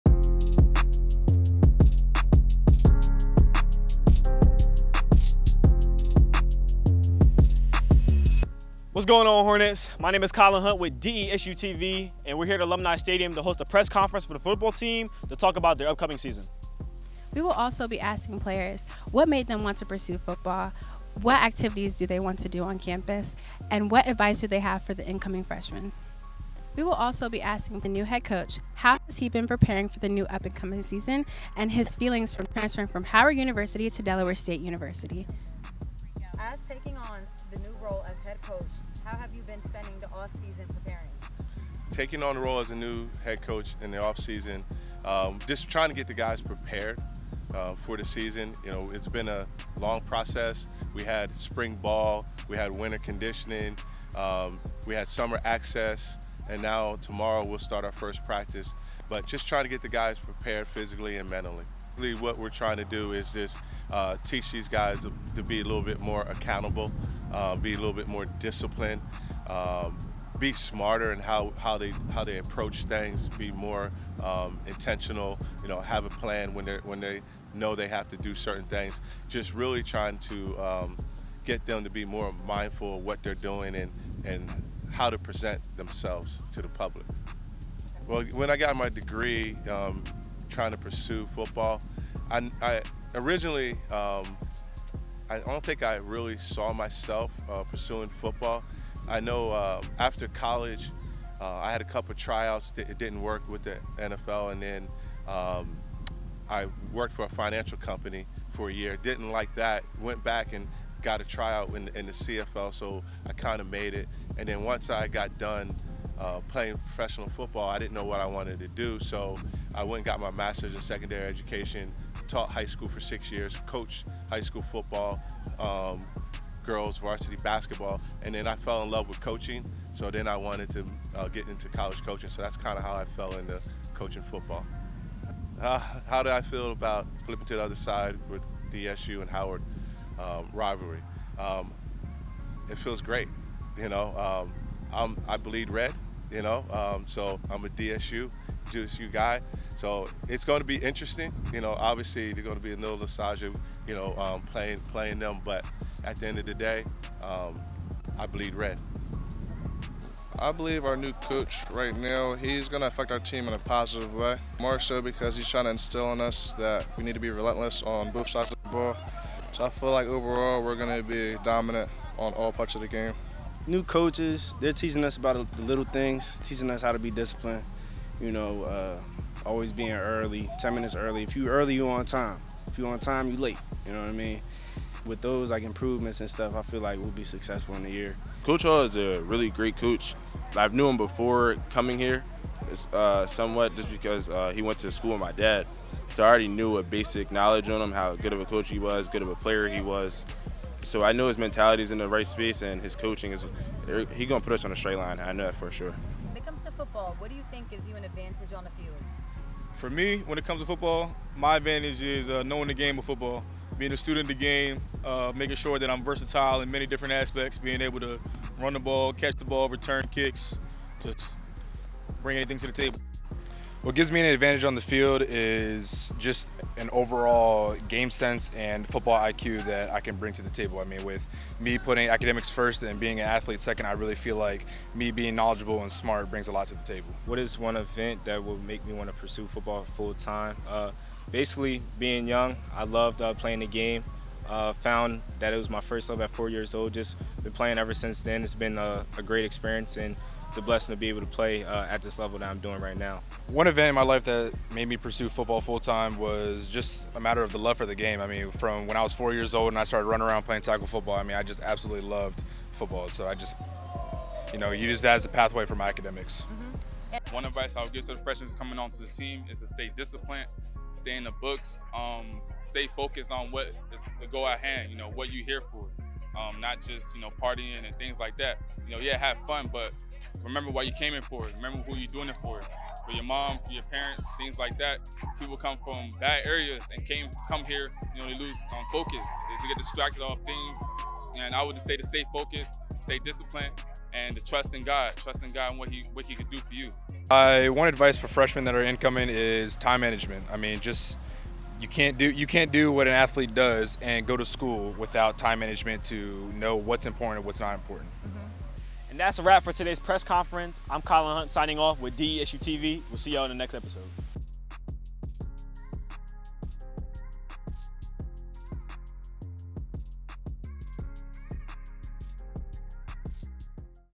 DSU Football Press Conference
What's up Hornets! today we hosted a press conference with the Delaware State Football Team to ask them a couple questions about their upcoming season this fall 2023 Semester. We got a chance to speak to the new head coach Lee Hull to talk about his vision for the football team, what his goals are, and how he feels about becoming a hornet after coaching at Howard University.